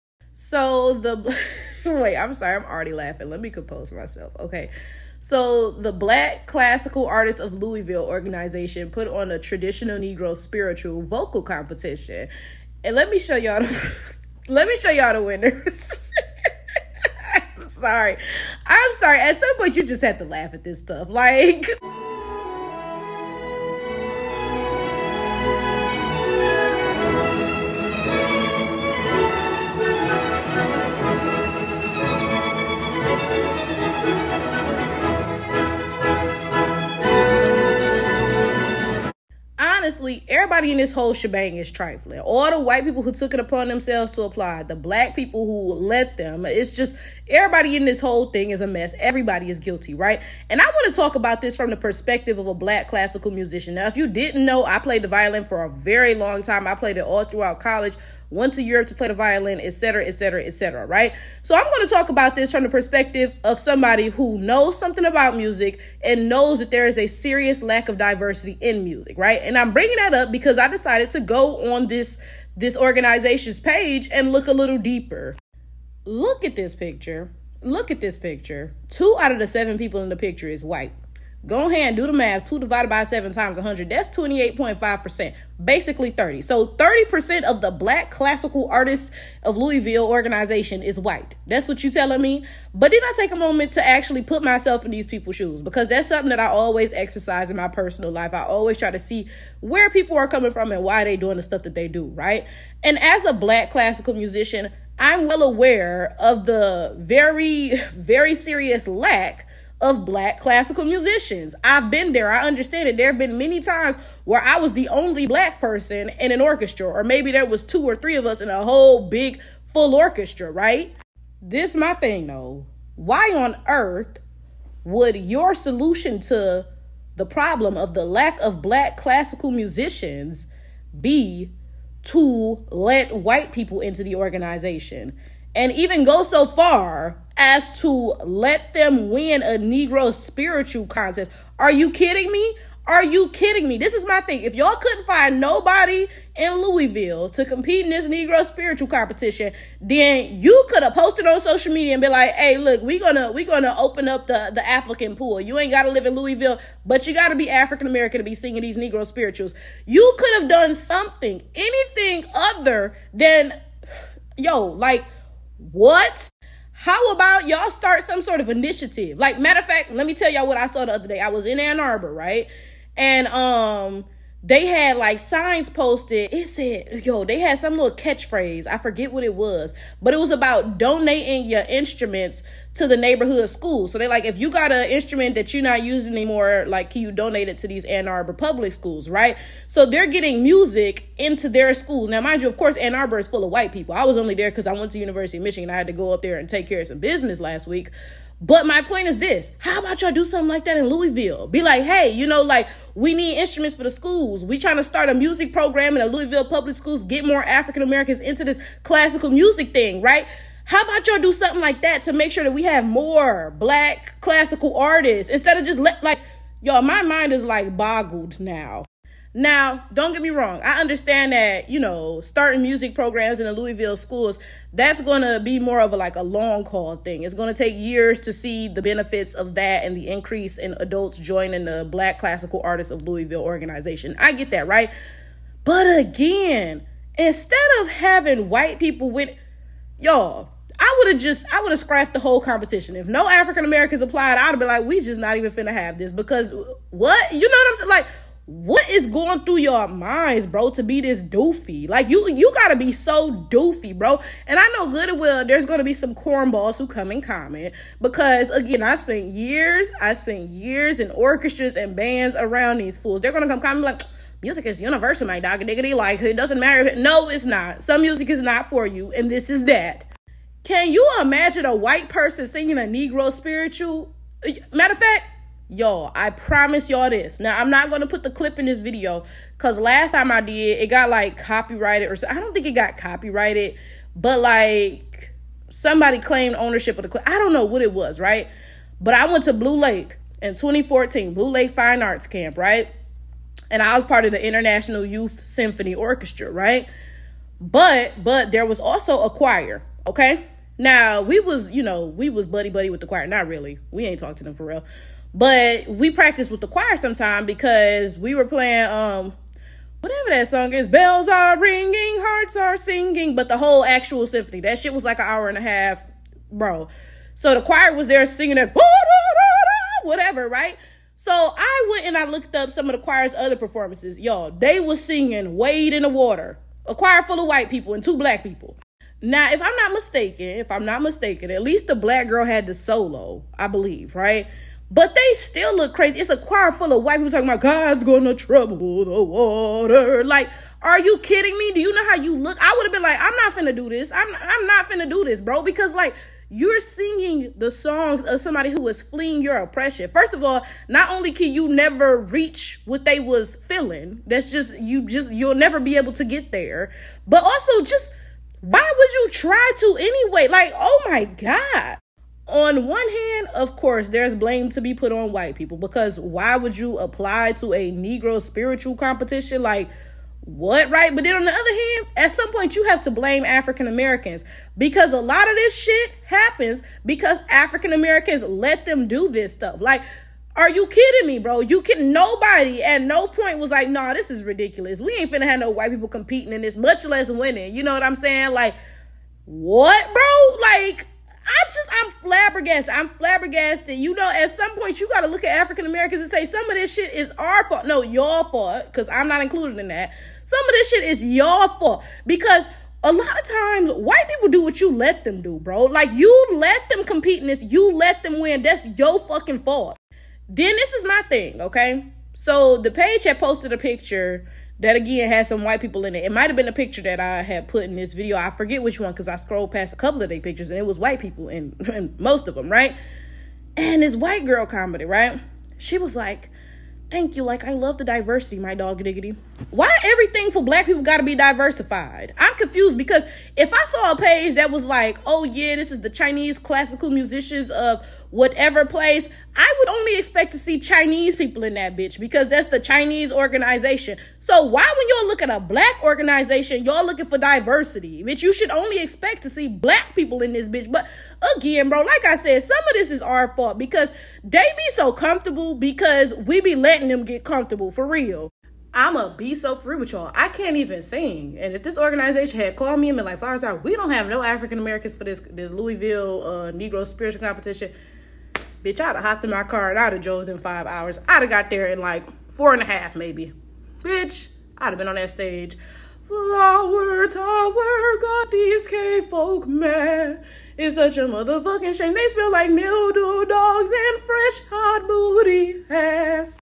The winners of the Traditional Negro Spiritual Voice Competition are... white?
Become a channel member for early uploads!